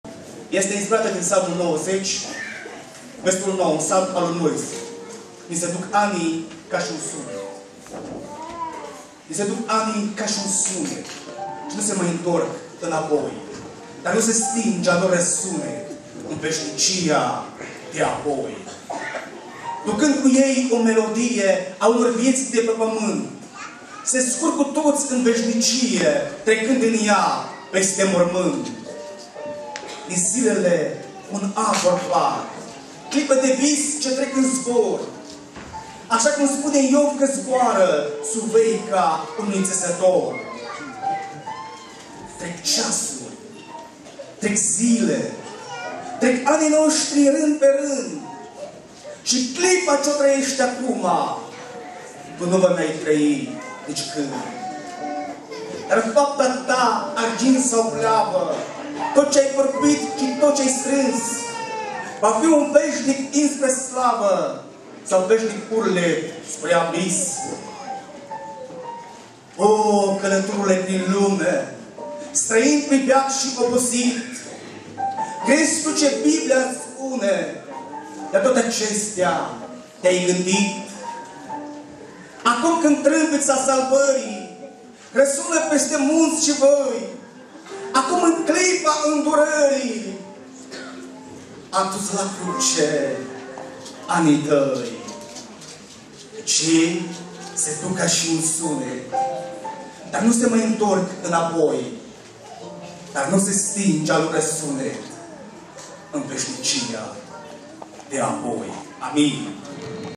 Marcel Sîmbuan - Poezie
Marcel Sîmbuan - Poezie. In biserica Muntele Sionului,din Arganda del Rey,Madrid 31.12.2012